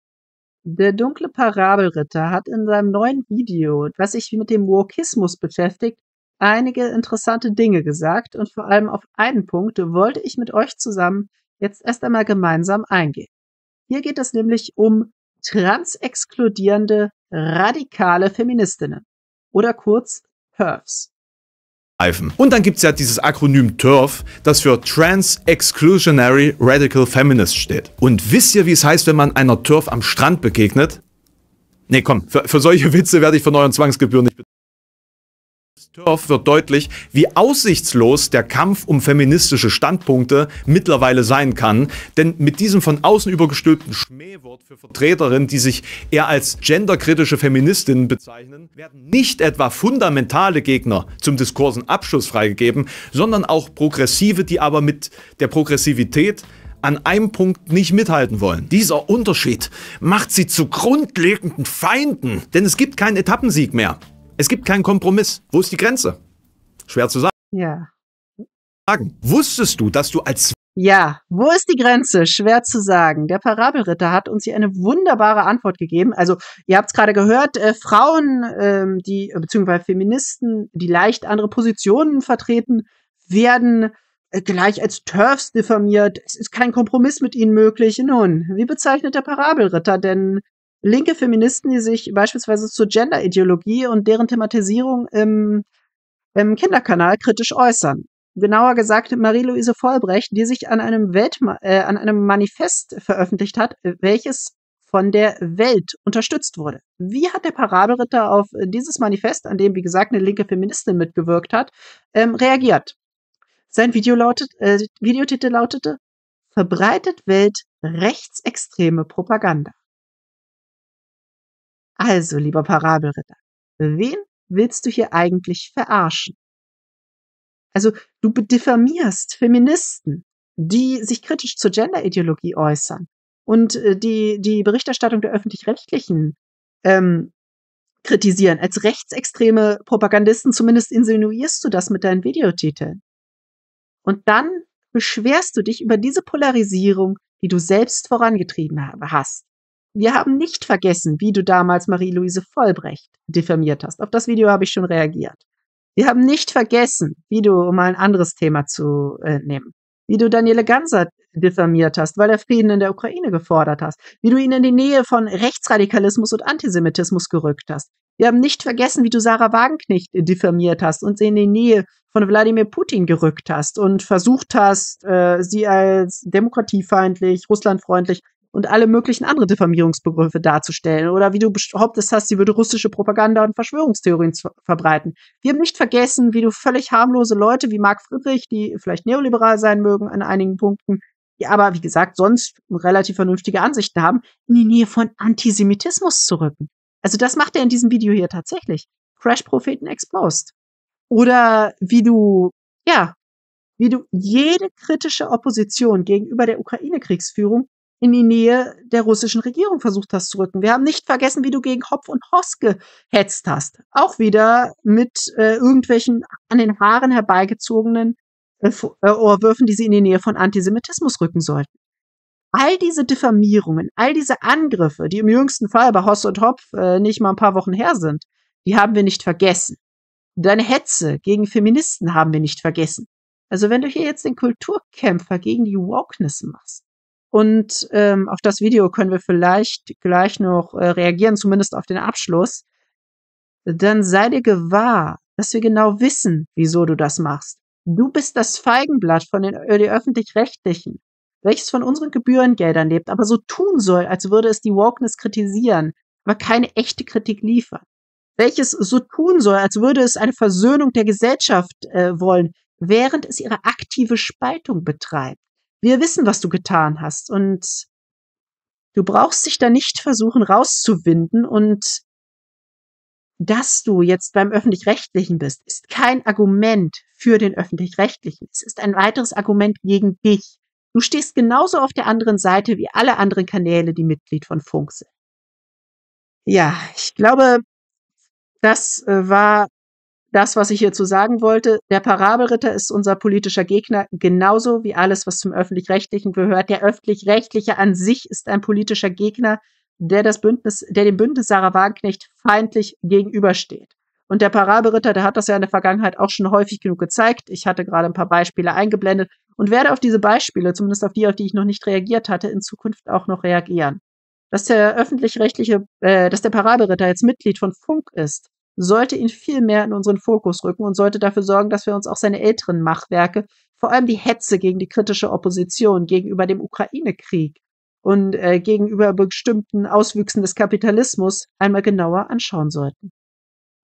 Das Feigenblatt des ÖRR! Der Parabelritter ist angeblich gegen Spaltung...
Der Dunkle Parabelritter über das Ende des Wokismus